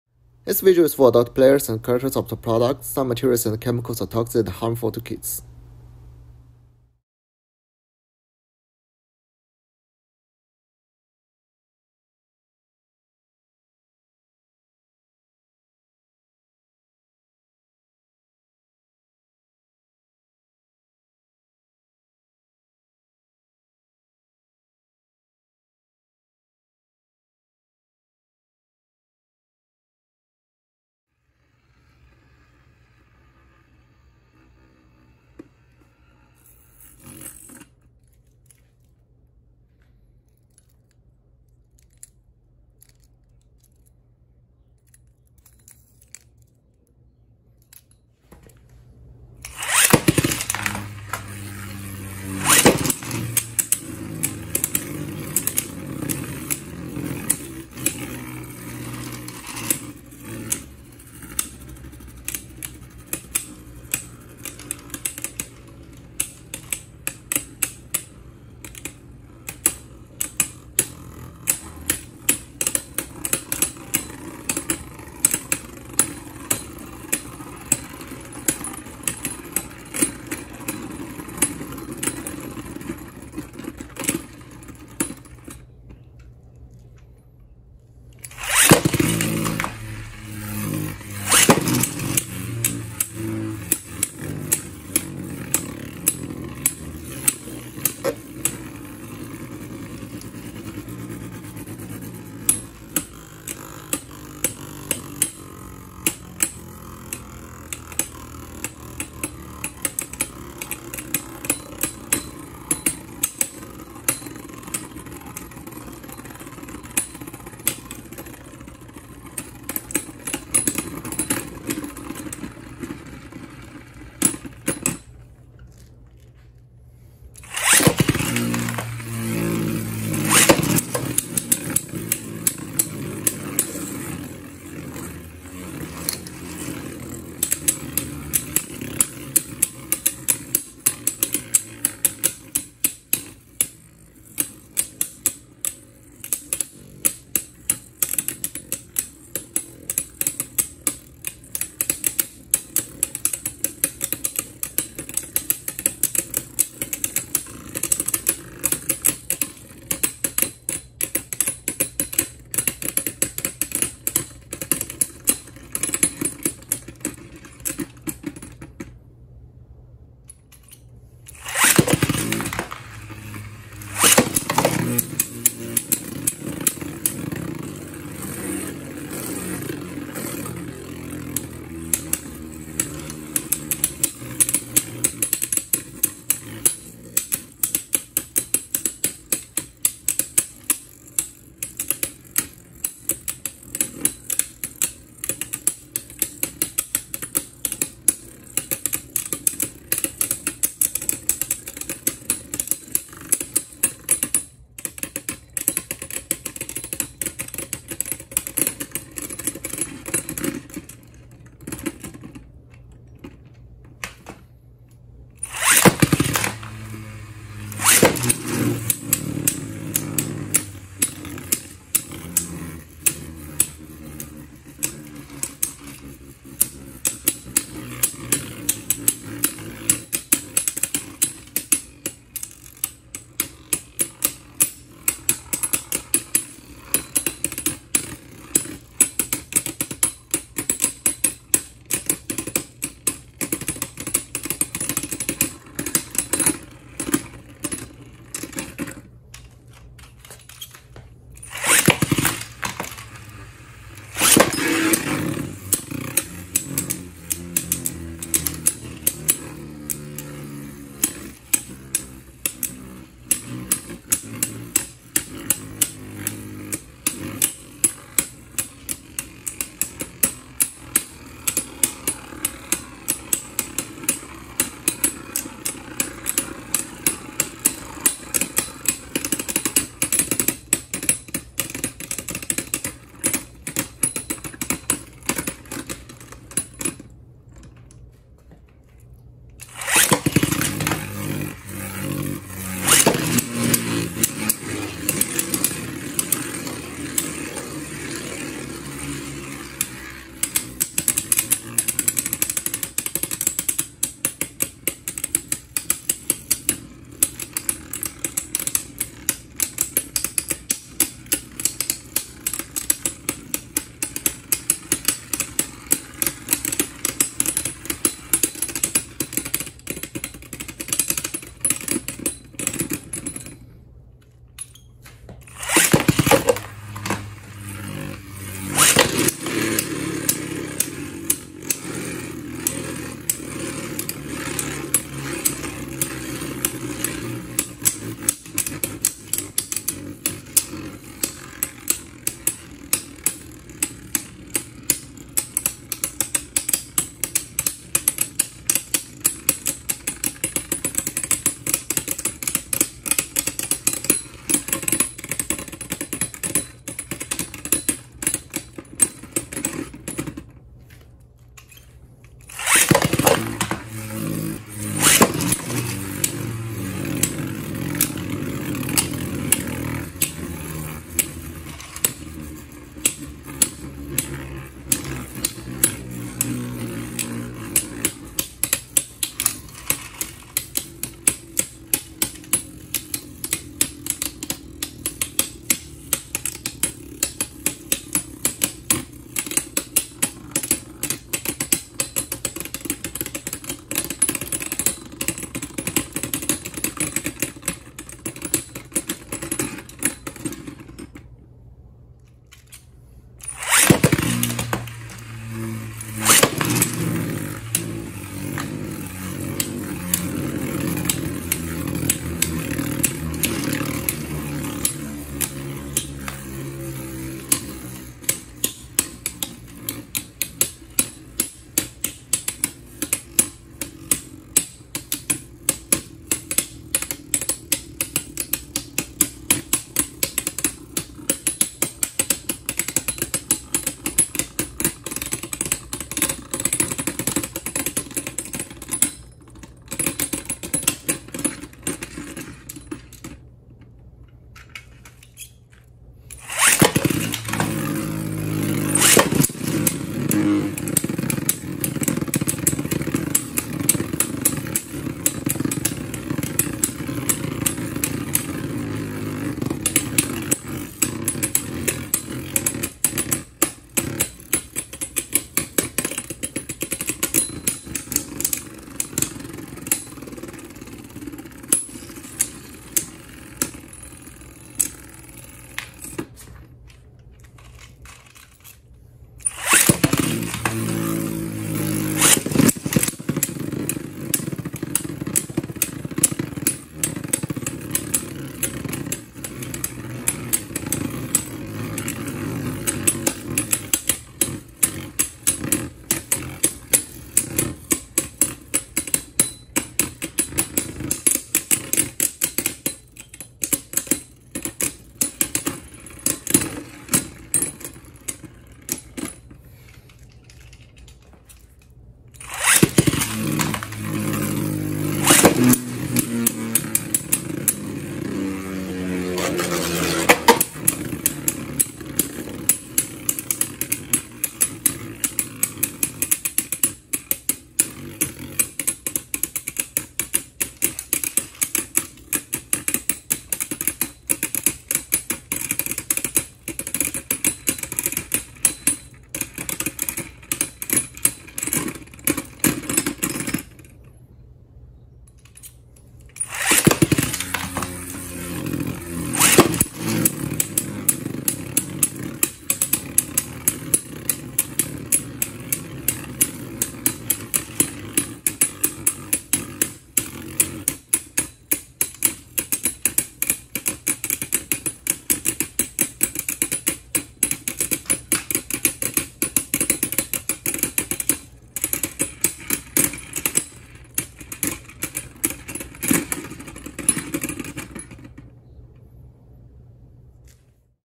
The Ultimate Diving - Despair Chaos. Tp. Br’-2 - Customized Beyblade / Beyblade Burst BU
My design of BU evolution for Deep Chaos.